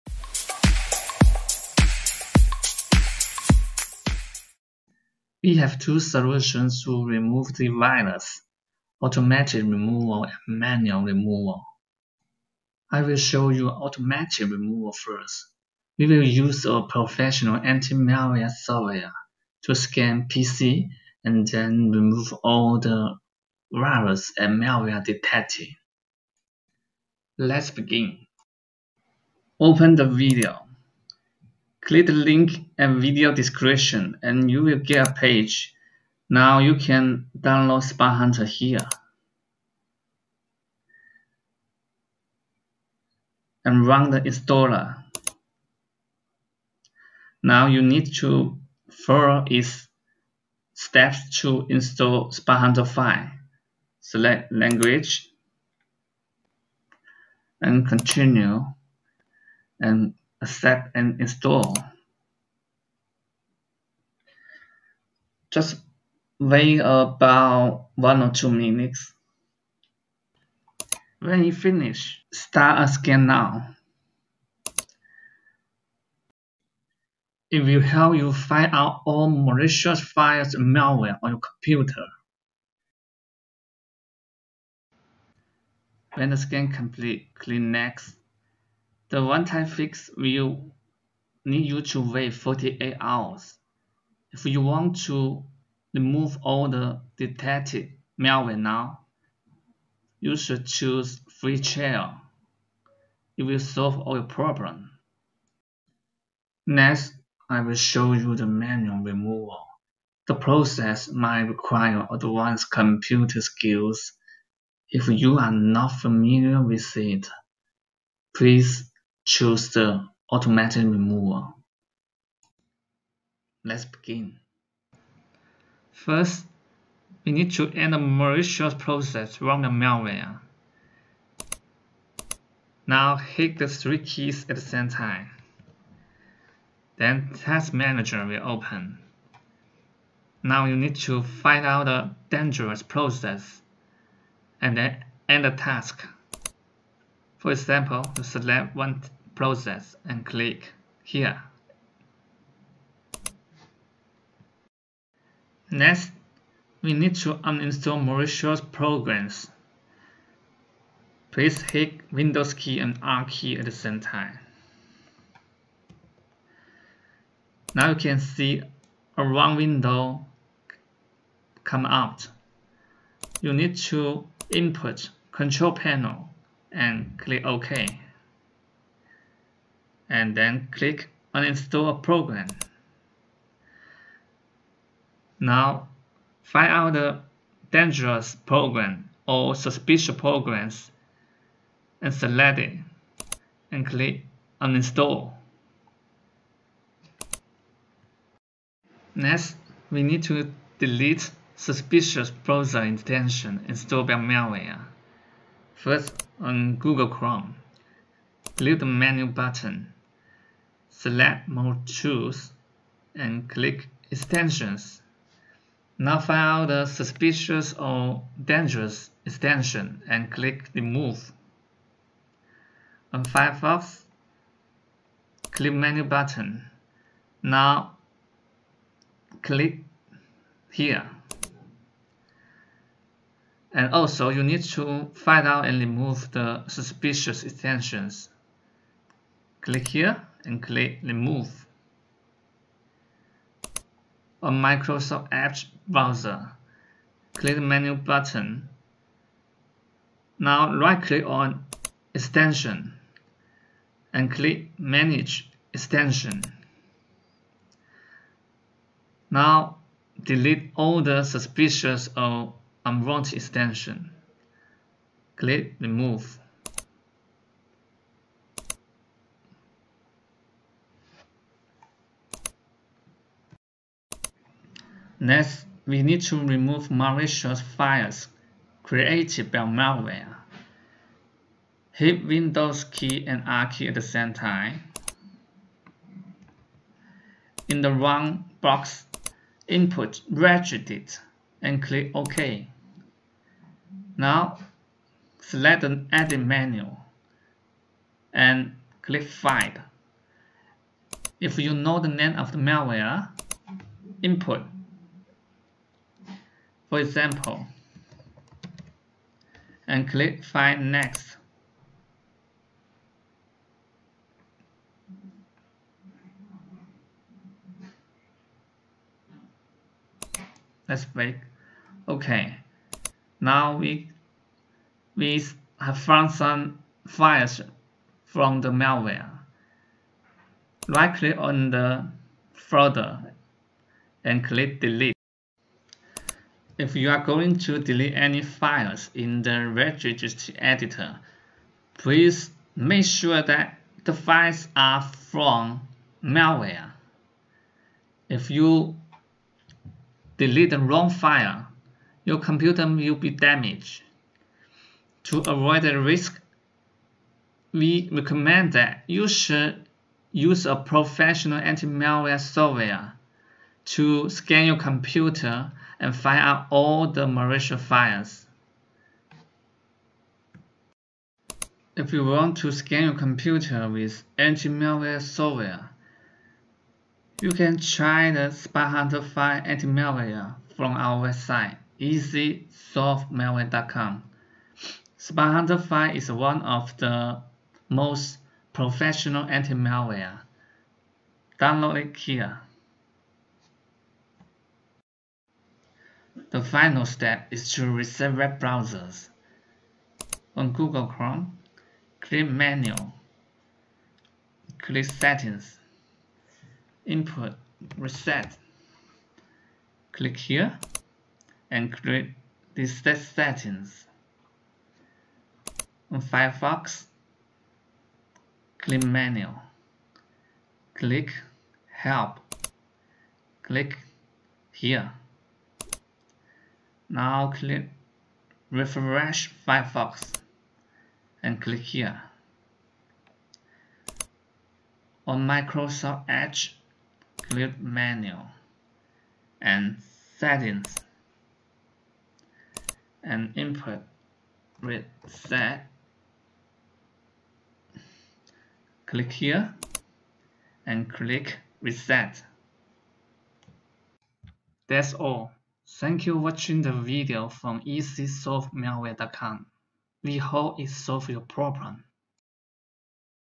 Backdoor:Win32/Bladabindi!ml Complete Removal (2023 update)
This video will show you how to remove Backdoor:Win32/Bladabindi!ml.